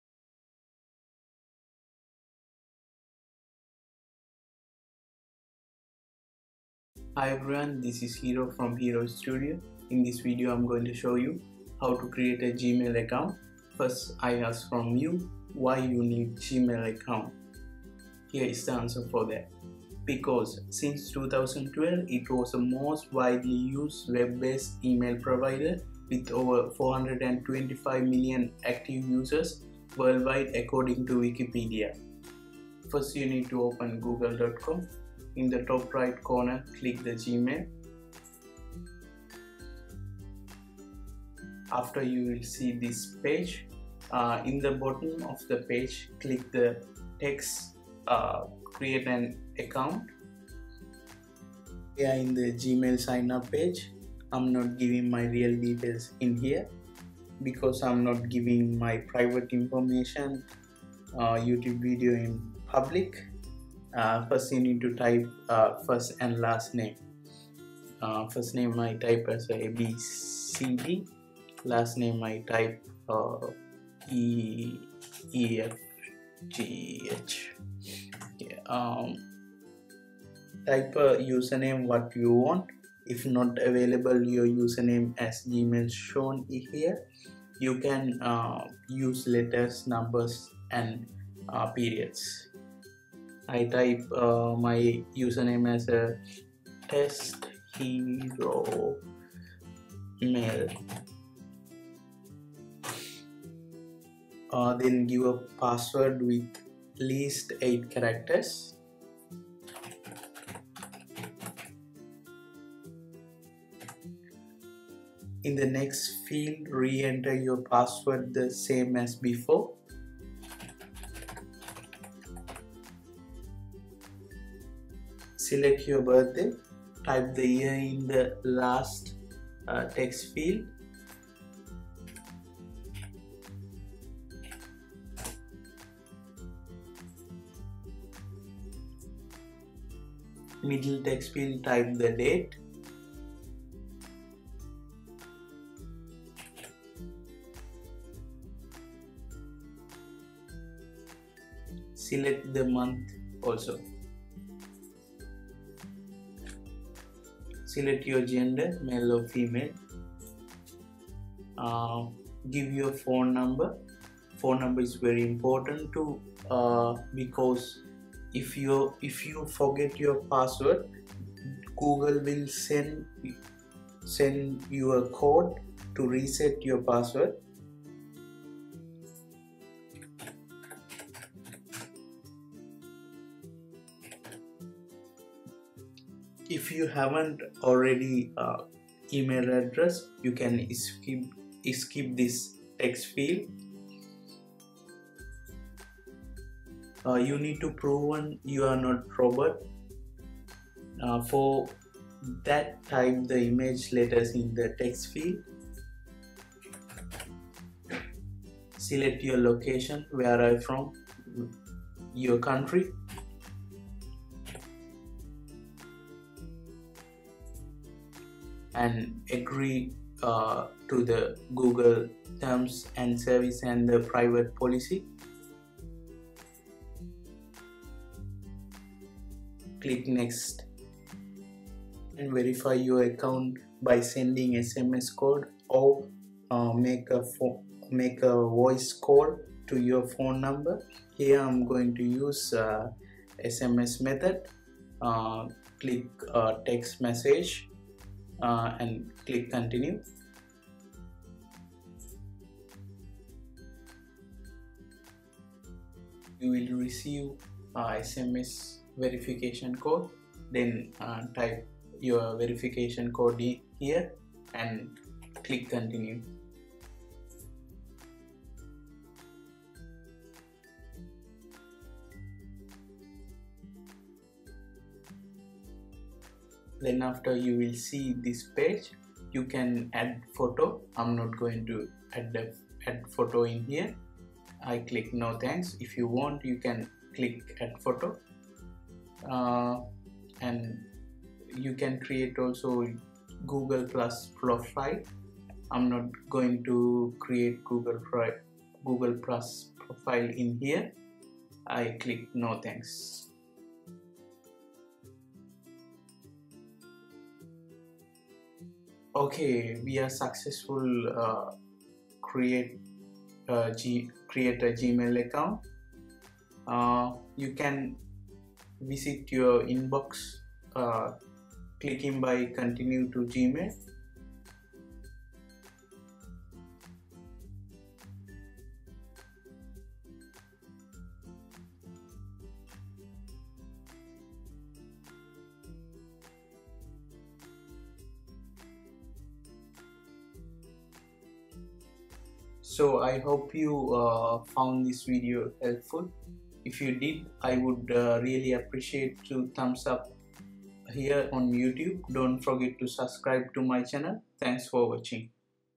How to create a Gmail account | How To Make A Gmail
This video demonstrate step by step to create a Google account | How To Make A Google Account .

Since 2012 Gmail was the most widely used web-bases email provider with over 425 million active users wordwide according Wikipedia .

Published by Hero from Hero Studio